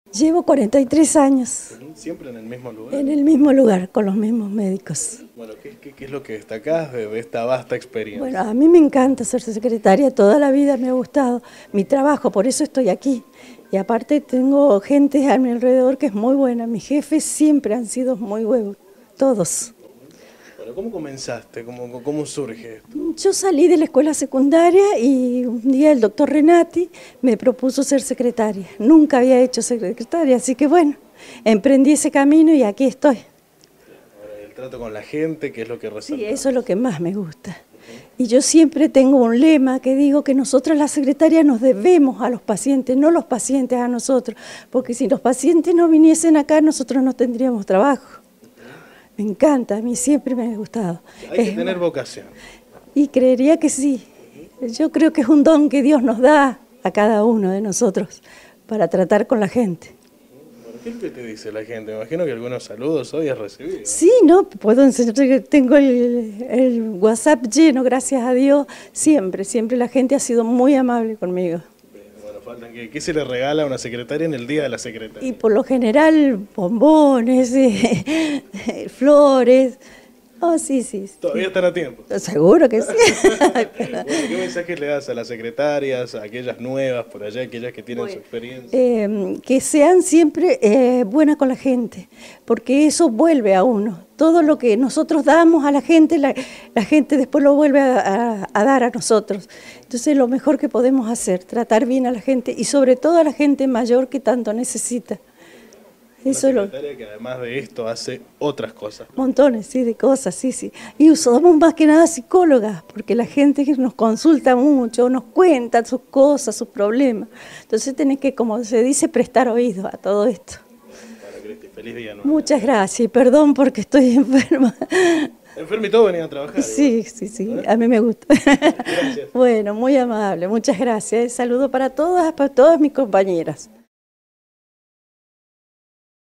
4 DE SEPTIEMBRE: DIA DE LA SECRETARIA
En este 4 de septiembre, Día de la Secretaria, Noticiero Andino decidió homenajear a través del testimonio de una de las secretarias más reconocidas y queridas del departamento, Cristina Ávalos, una vida al servicio de la comunidad en un consultorio cardiológico.